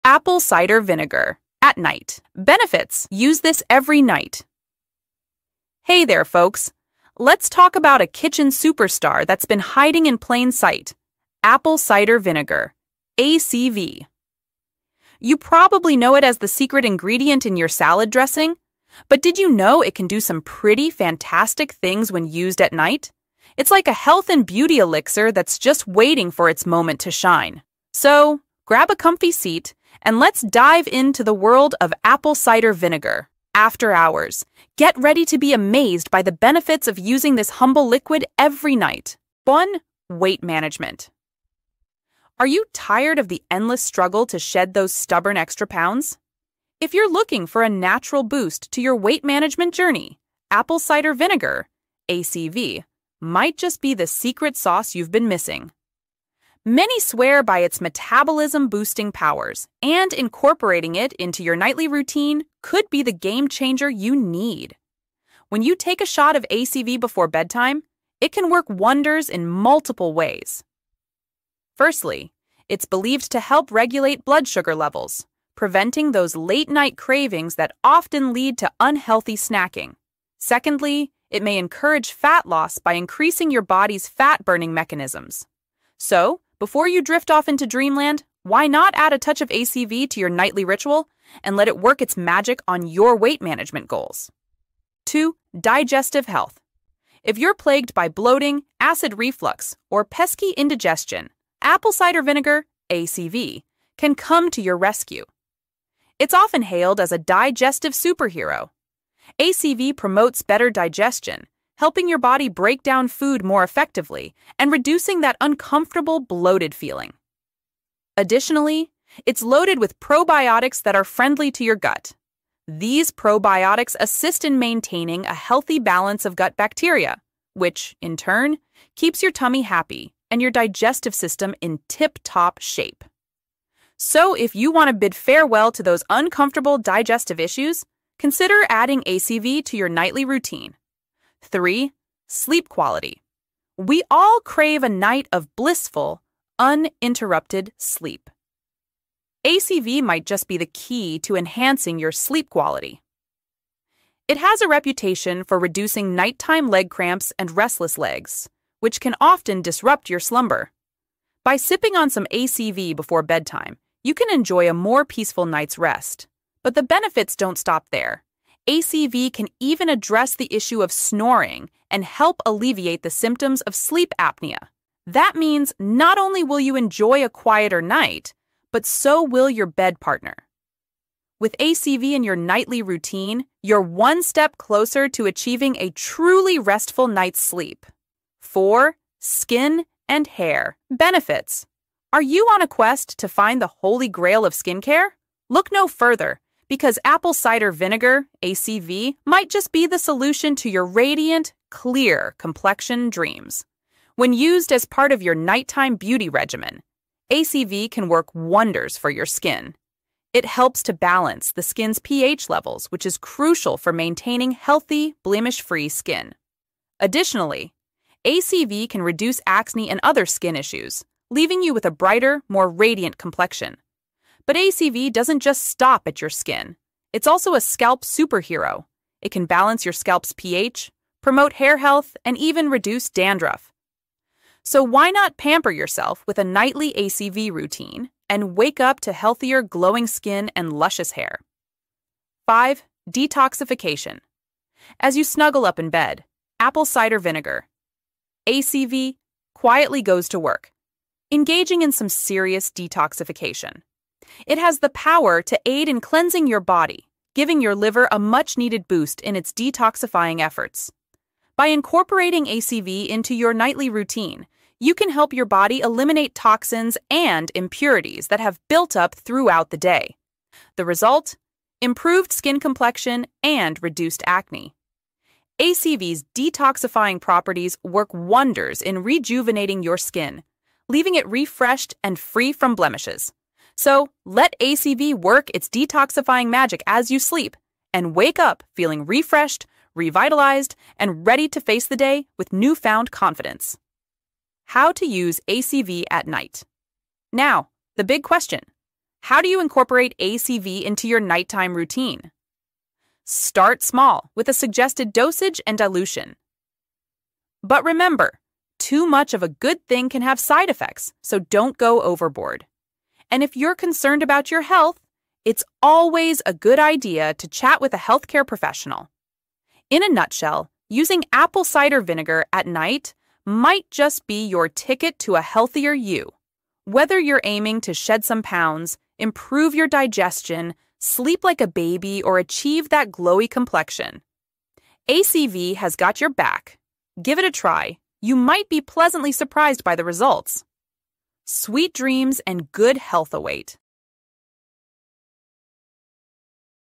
Apple Cider Vinegar At NIGHT Benefits (Use This Every Night) - HEALTHPECIAL
Apple Cider Vinegar At NIGHT Benefits (Use This Every Night)

Want better sleep, clearer skin, and a healthier you? Dive into the benefits of using ACV at night in this video! From weight management to improved digestion and hair care, find out how this simple routine might change your life. Learn the dosages, precautions, and tips to make ACV your nightly companion. Join the ACV craze and unlock a world of wellness while you catch those Z's! Don't snooze on this – hit play and discover the secret sauce to a better bedtime routine!

Recommended Products for Your Daily Vitamin

Vitamin A

Vitamin B

Vitamin C

Vitamin D3

Fish Oil